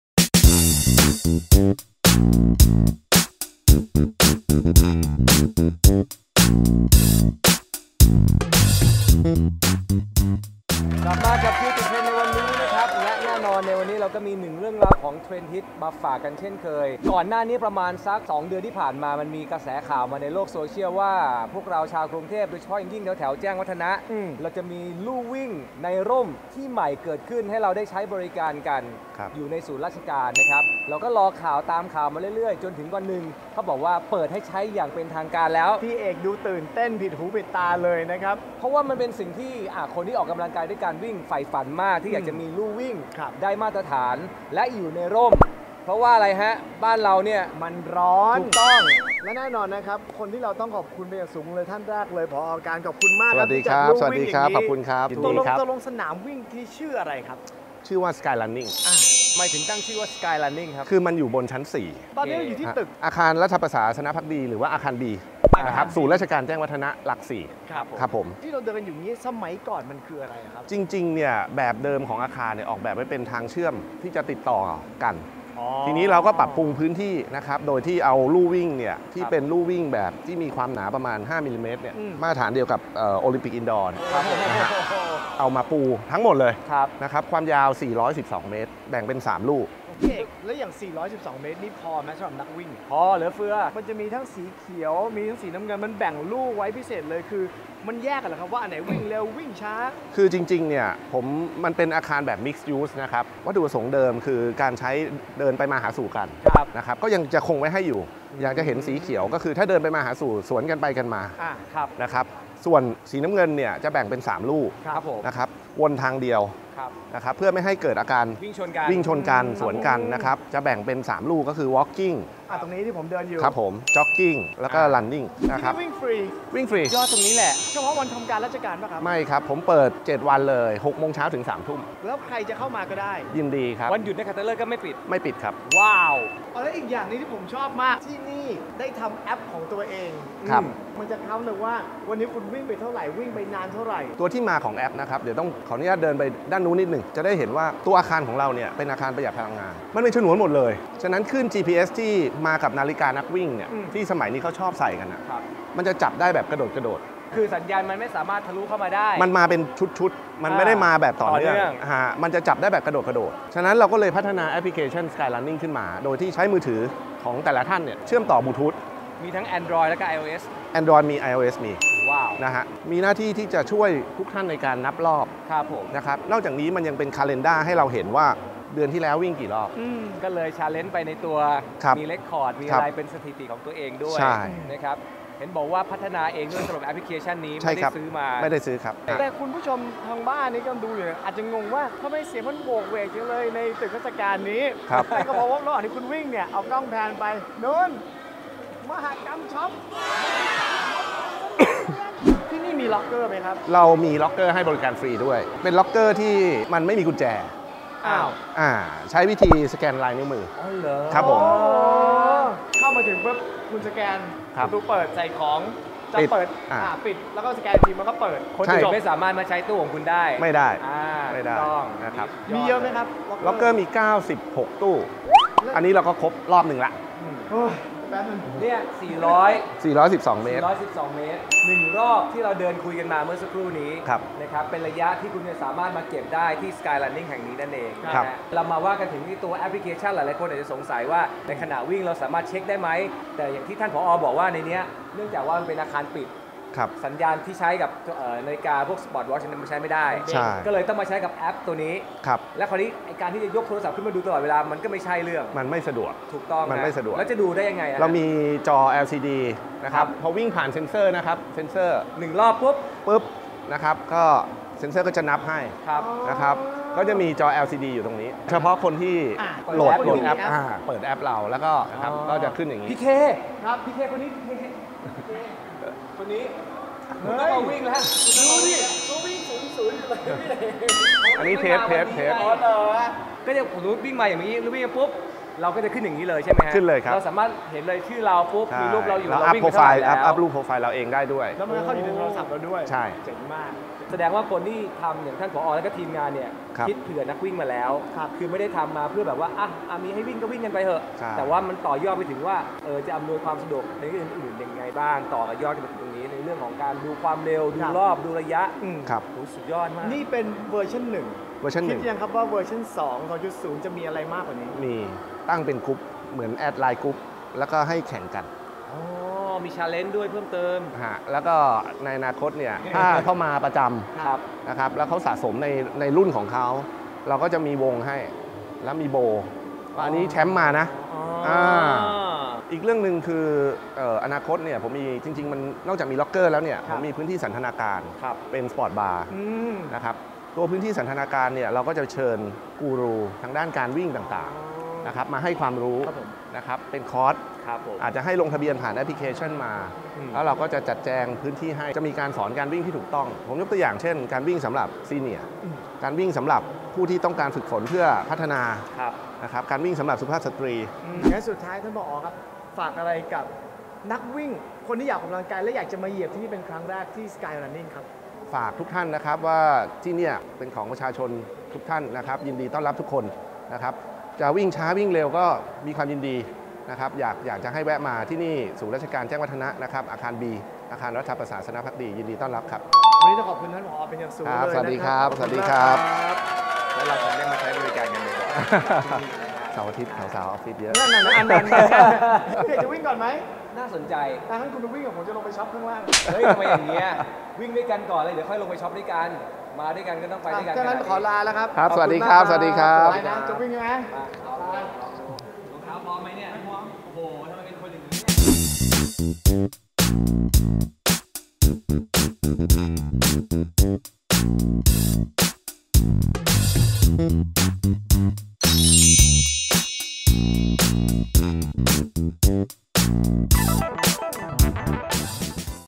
Chillติดเทรนด์ EP.14 Sky Running
Chillติดเทรนด์ อีพีนี้เอาใจเหล่านักวิ่งเป็นพิเศษเลย เพราะเราพามาที่
Sky Running ชั้น 4 อาคารรัฐประศาสนภักดี (อาคารบี) ศูนย์ราชการฯ แจ้งวัฒนะ
ข้างในจะเป็นยังไง และจะมีอะไรพิเศษๆ บ้าง ตามมาดูกันเลย!!!